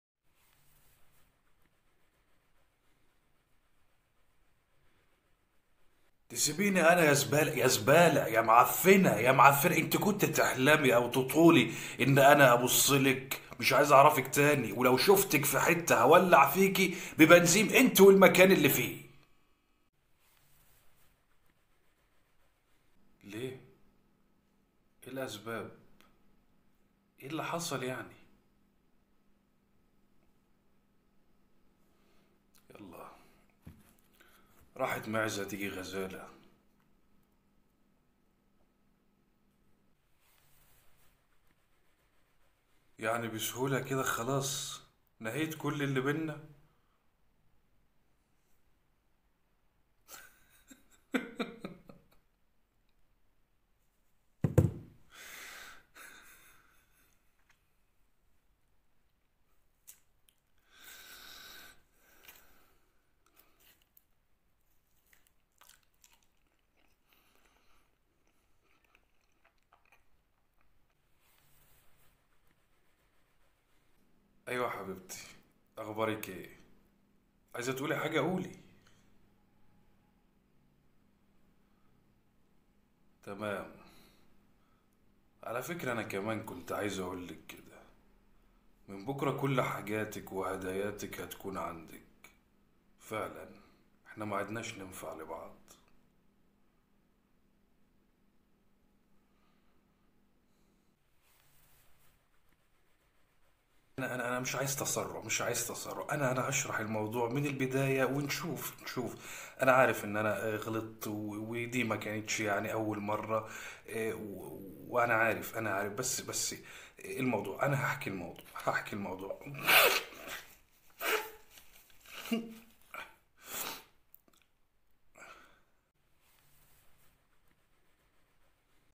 رد فعل كل برج لما حبيبة يديلة صابونة 🤣!؟ فانتزيا الأبراج حلقة مسخررة🤣🤣
@اسأل محمد وعيلته
صفحتنا عالفيسبوك علشان تشوفوا فيديوهتنا وتحضرو الايف معانا

_link